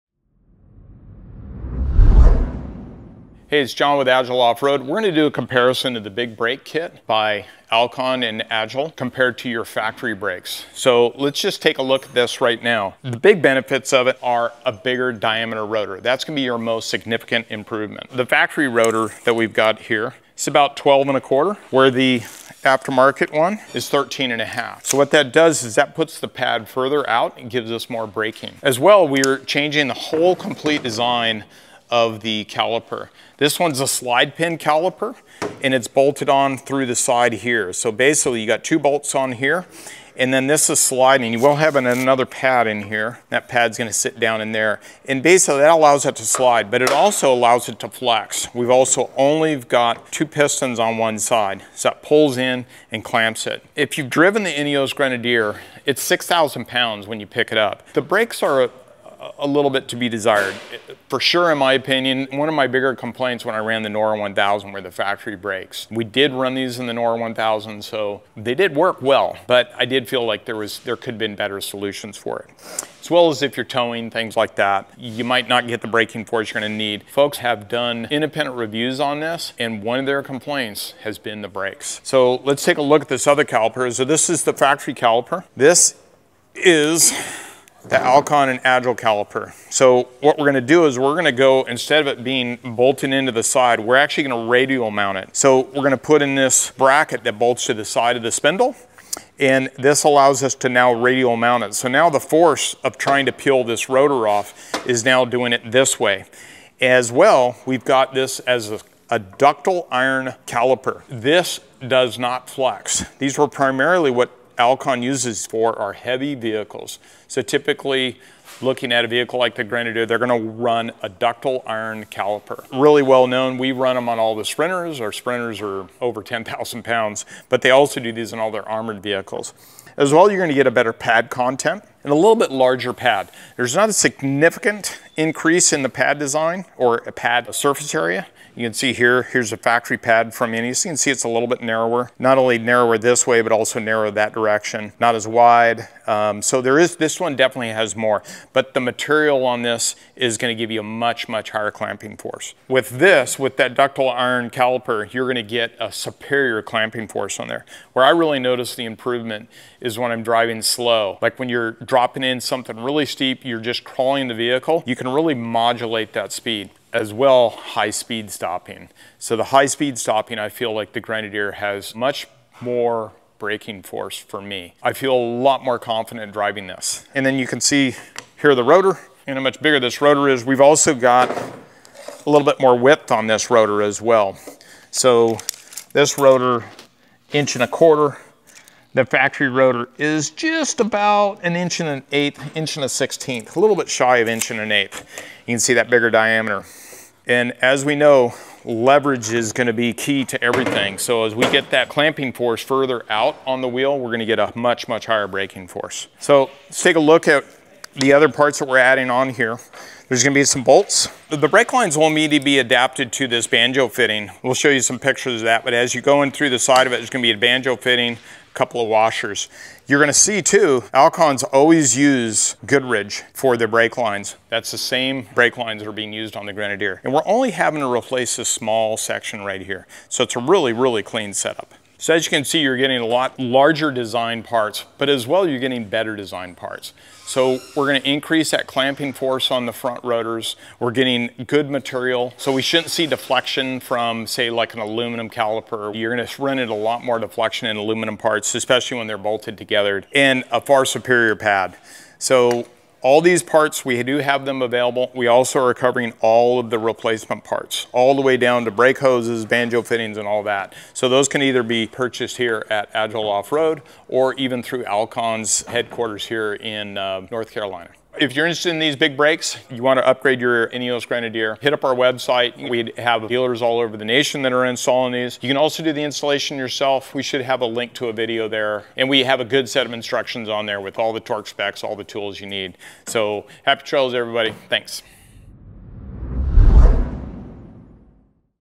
Grenadier Brake Showdown: Alcon vs. Factory
Better stopping power starts with better engineering.
In this video, John breaks down the key differences between the factory brake setup on the INEOS Grenadier and the Alcon Big Brake Kit we install at Agile Offroad. From rotor size and caliper design to thermal capacity and pad compound, John explains exactly why the Alcon system delivers stronger braking, improved fade resistance, and better pedal feel — both on-road and off-road.

🔧 In this video:
• Alcon vs. factory brake construction and materials
• Why caliper stiffness and rotor mass matter
• Real-world performance differences you can feel
• Maintenance and long-term reliability insights

Whether you’re towing, trail driving, or just want more confidence behind the wheel, this upgrade is built to perform where it counts.